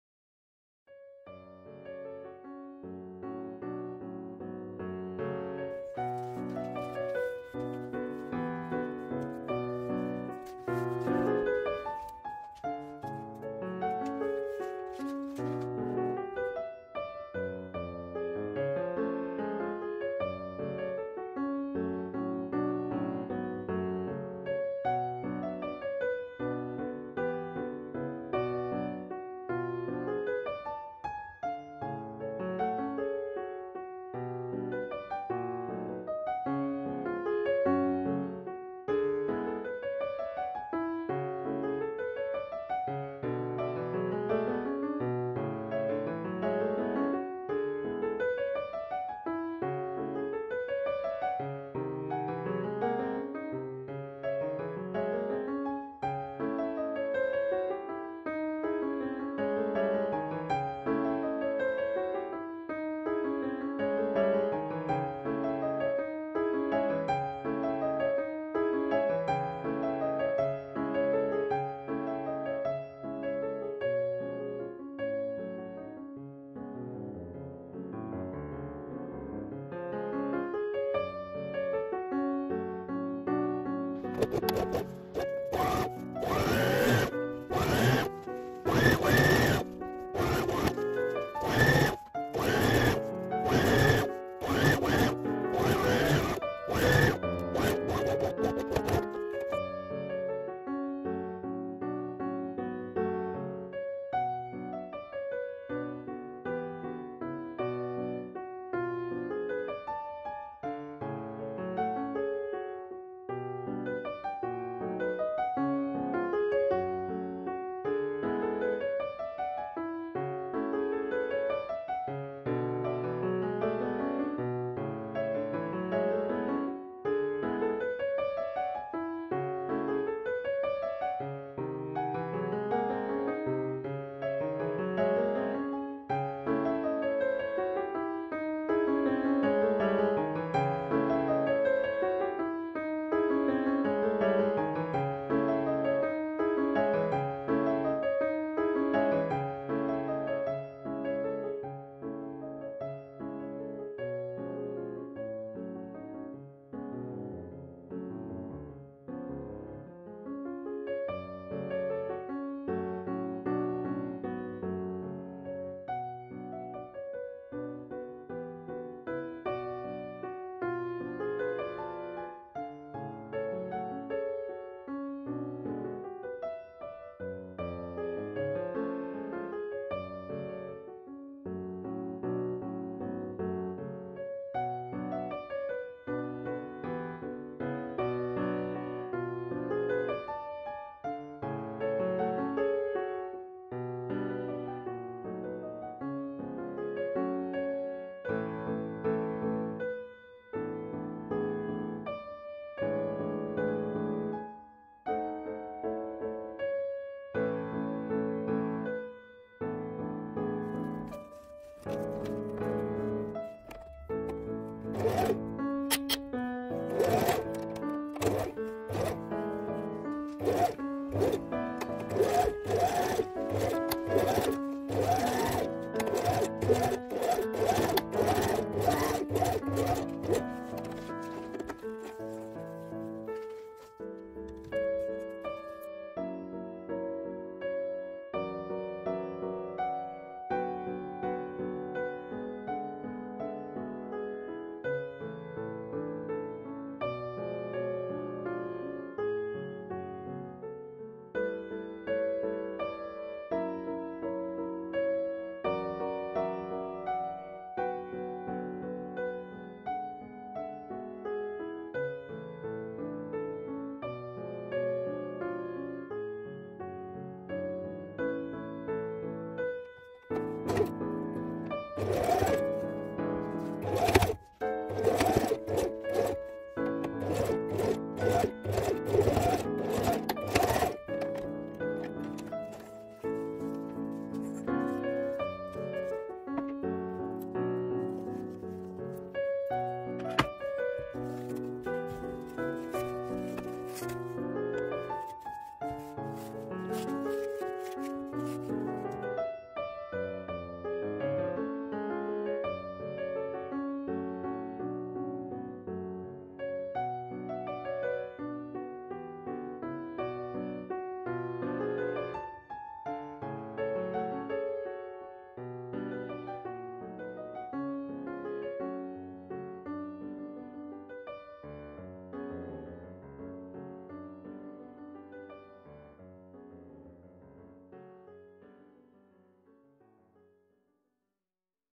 DIY Beautiful Wide Elastic Headband with Ruffle Scrunchie Pattern | How to Make Ruffle Hair Band
Very Easy! Learn how to make headband with cloth and elastic with ruffle scrunchie pattern. It is a great style to have. Enjoy making🌹🌹🌹